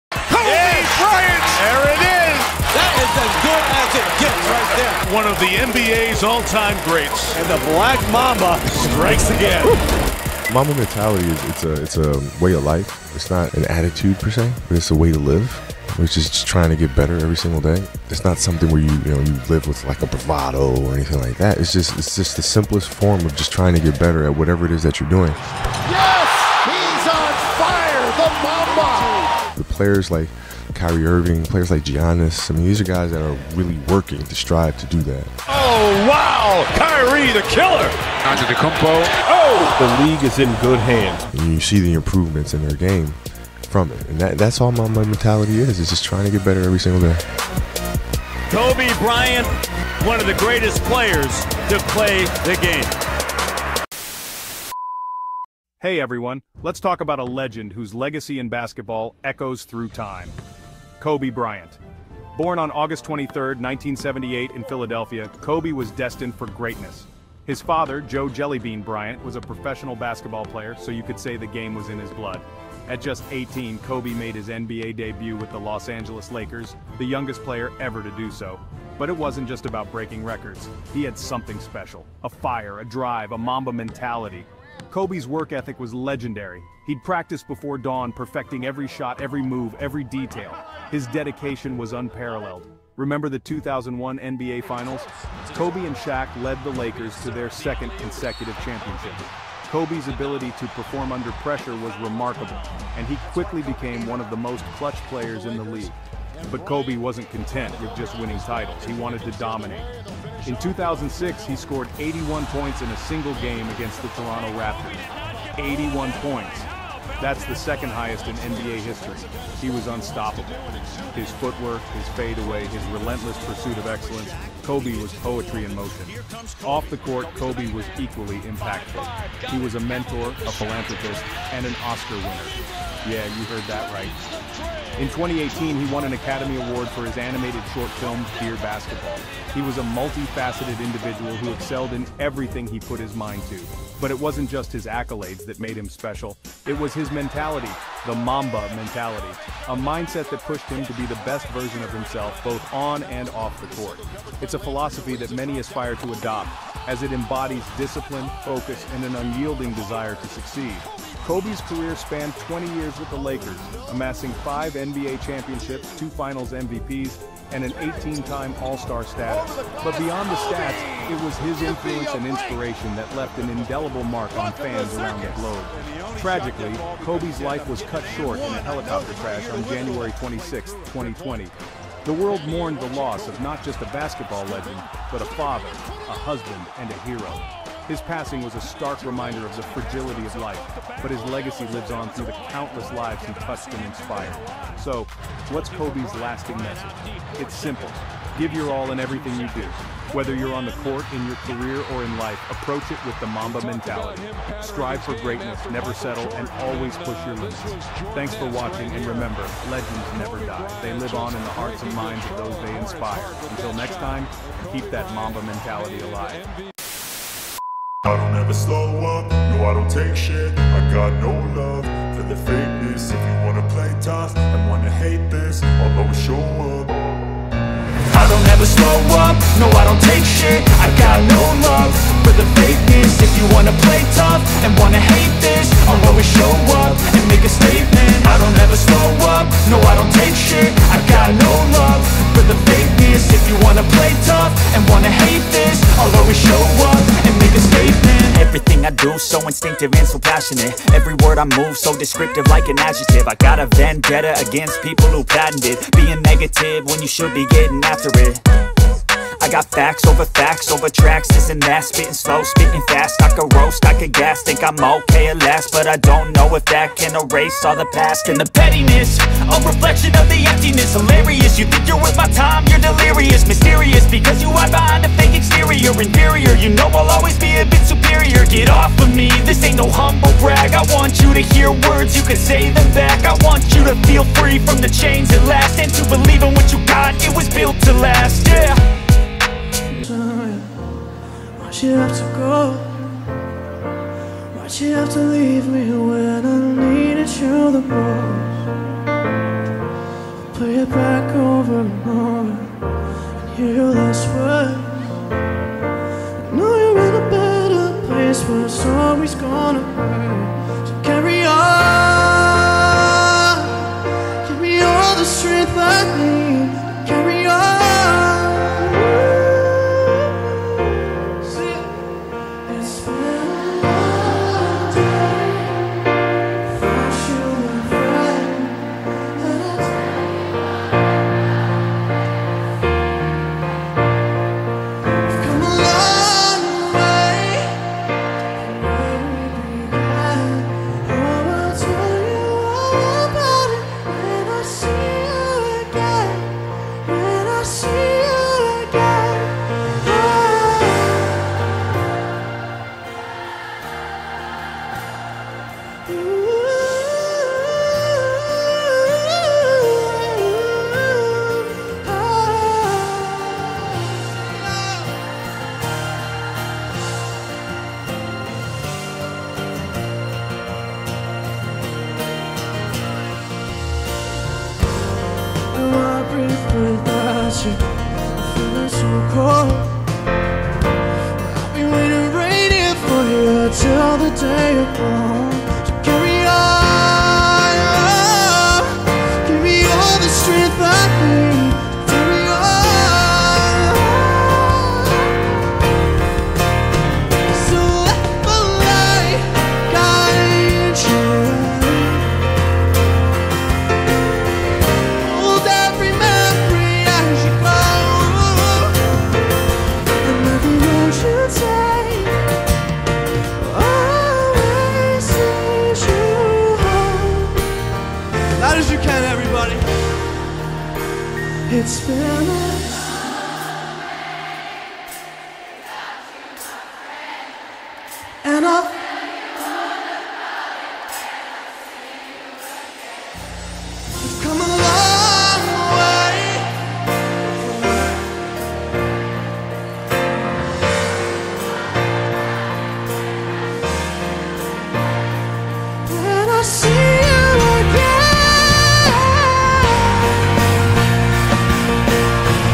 Kobe Bryant: The Legacy That Changed Basketball Forever
This video is a tribute to the legendary Kobe Bryant, one of the greatest basketball players of all time. We honor his incredible career, his fierce competitive spirit, and his lasting impact on the game. Witness his iconic highlights, hear inspiring words, and relive the moments that cemented Kobe as a global icon.

LONG LIVE KOBE BRYANT
REST IN PEACE to Gianna Maria-Onor Bryant and ALL of the others that lost their lives in the helicopter crash!